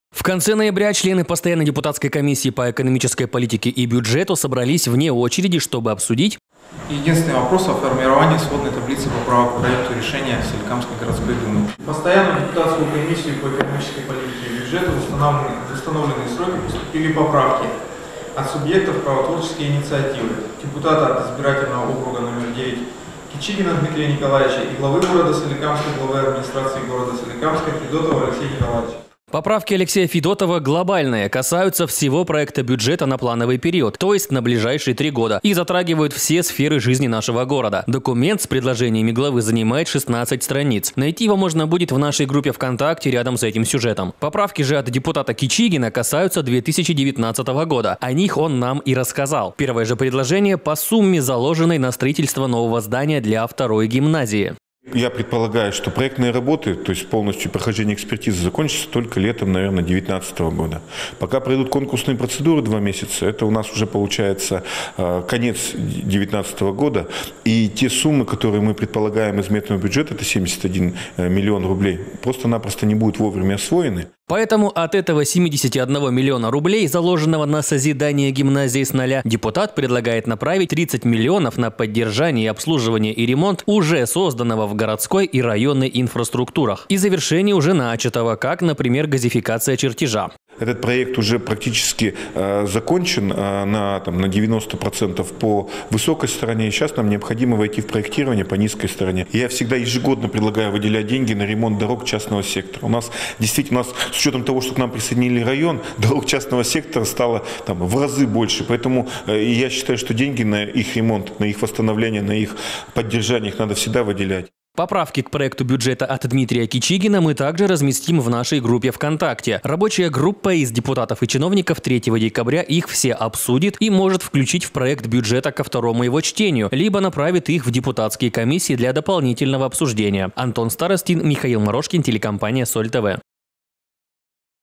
Поправки в бюджет_30.11.2018_СольТВ
Городская дума в ноябре в первом чтении одобрила бюджет Соликамского городского округа на 19ый и последующие годы. Пока не настал черёд второго чтения, в проект бюджета можно вносить поправки. И они уже есть. Подробней о них в нашем материале.